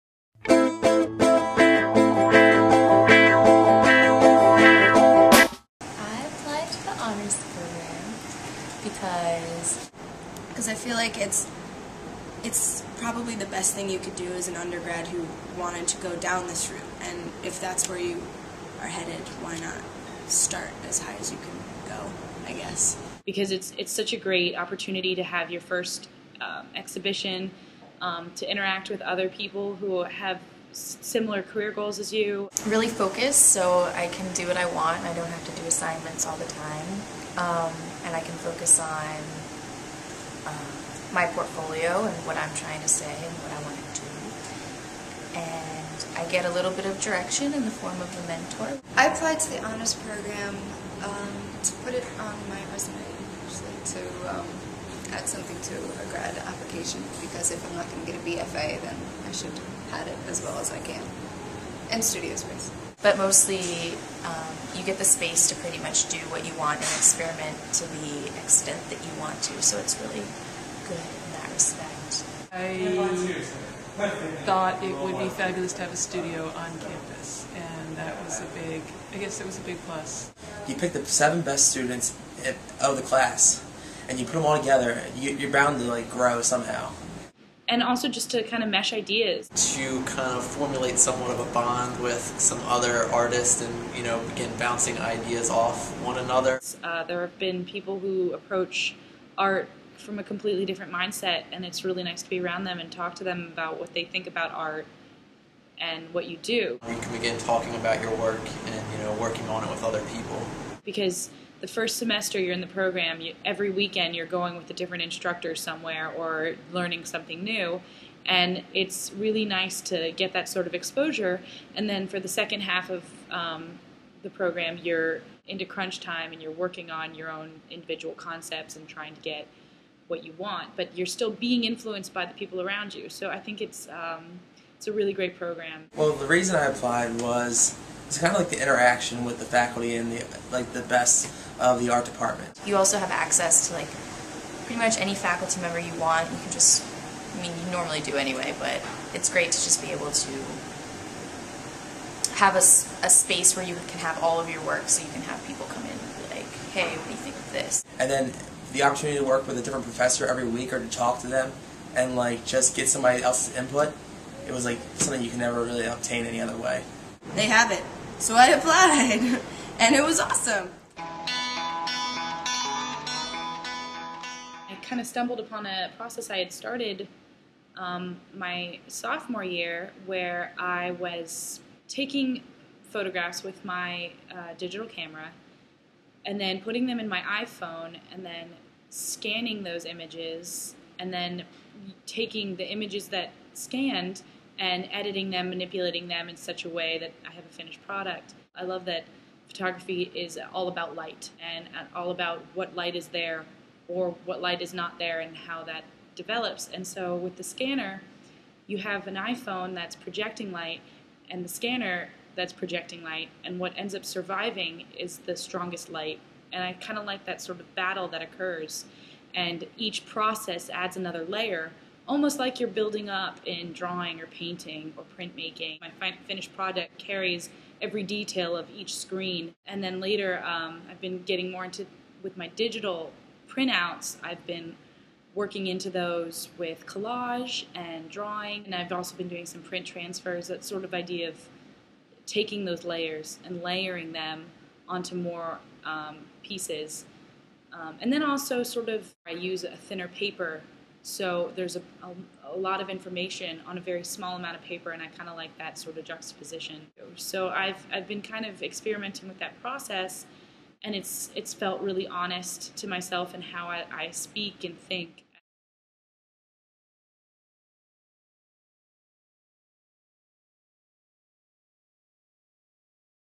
UMD Undergraduate Honors Program - Katie Friedgen Interview
A preview of the UMD Undergraduate Honors Program with the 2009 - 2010 Honors Program artists: Annie Kielman, Katie Friedgen, Jonathan Kellogg, Laura Cooke, Susan Chapin, Sarah Martin, and Anthony Comes.